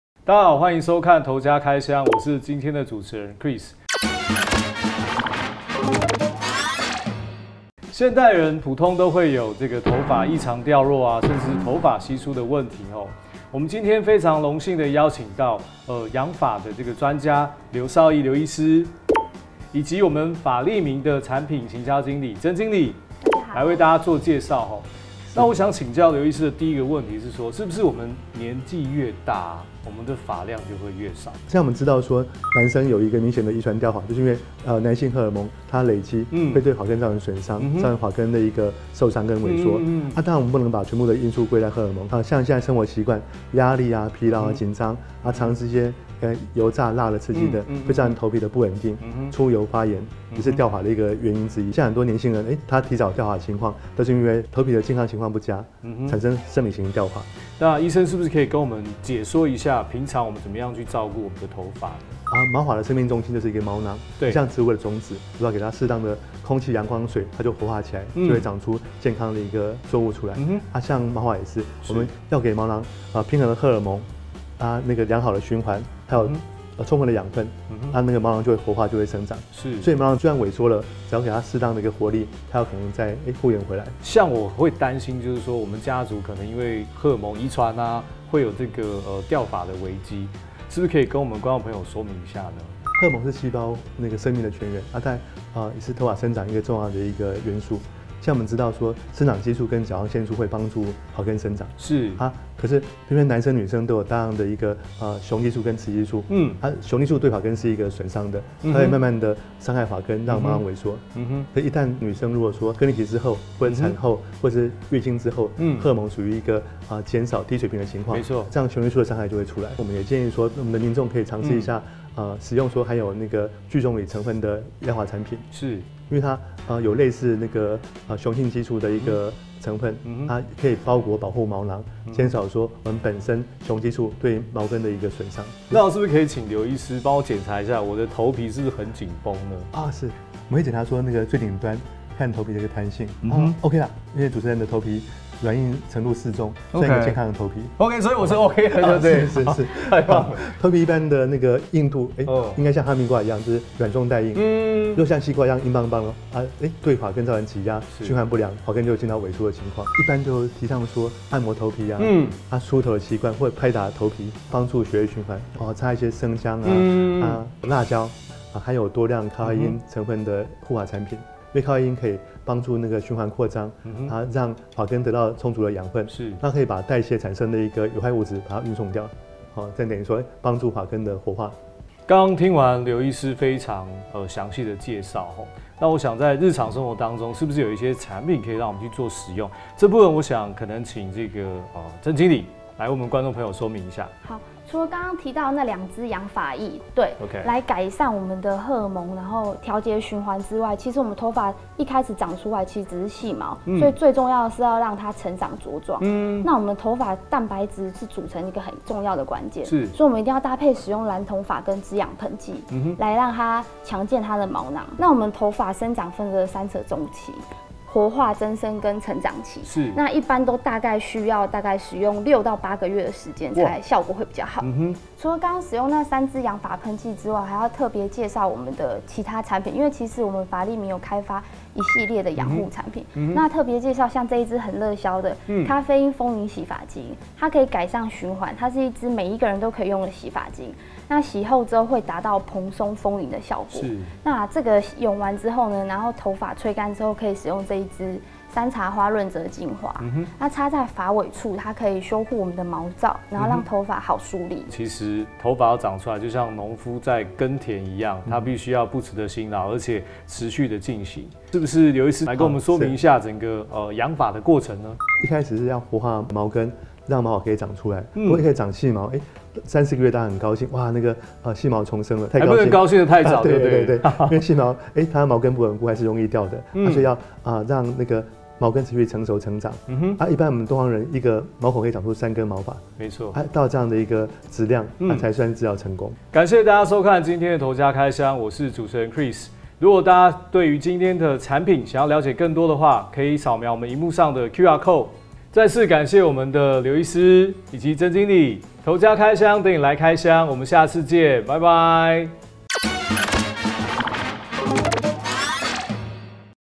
【頭家開箱×髮利明鋸棕櫚草本養髮液】醫生不說的生髮秘方！│台灣雷射藥妝國際網
由生髮門診專家--書田診所醫院劉紹毅醫師主講生髮的祕方
頭髮生長與賀爾蒙、循環、營養最有相關，解決這三個問題就可以讓頭髮健康成長。